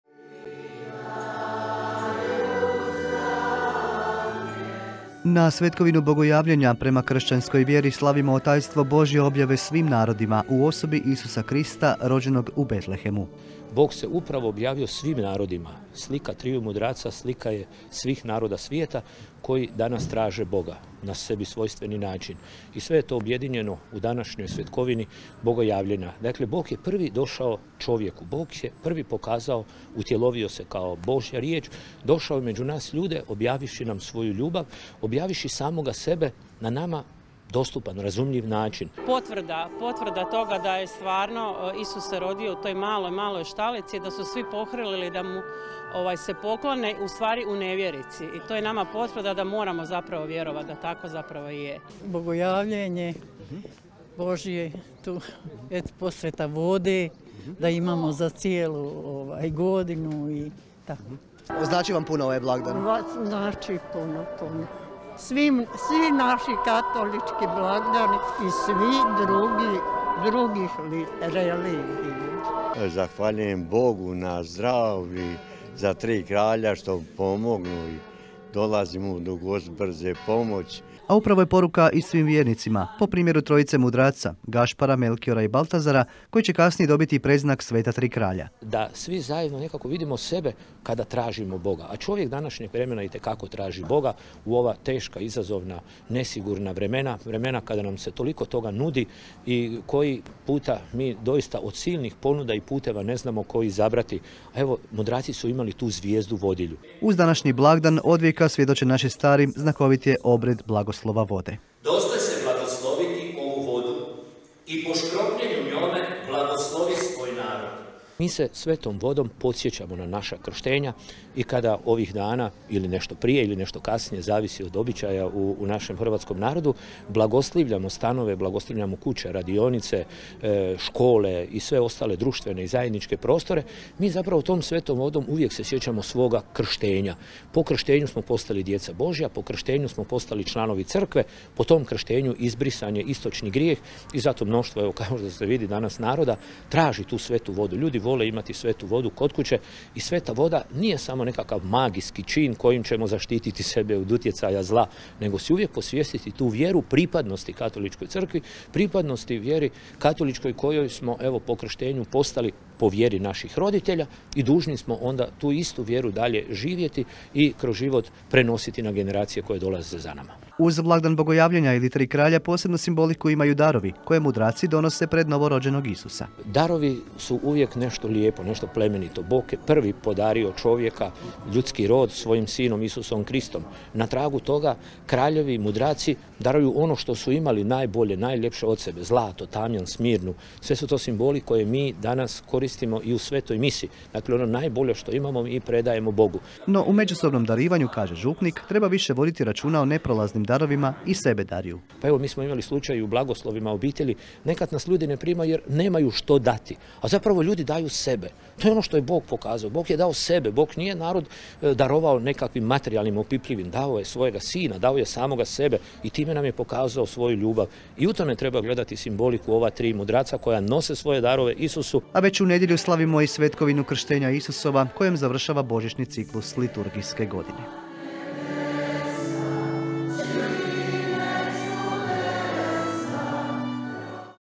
SBTV - DNEVNIK - SVETKOVINA BOGOJAVLJENJA – SVETA TRI KRALJA - 06.01.2023.
Katolički vjernici danas proslavljaju svetkovinu Bogojavljenja odnosno blagdan sveta tri kralja. Slavlje ovog blagdana ima početke u istočnim kršćanskim crkvama, a uključivalo je Isusovo rođenje, pohod trojice mudraca u Betlehem, te sve događaje Isusova djetinjstva do njegova krštenja. Slavi se uz tradicionalni obred blagoslova vode.

DNEVNIK SBTV-a | PON-PET | 20:00 i 22:00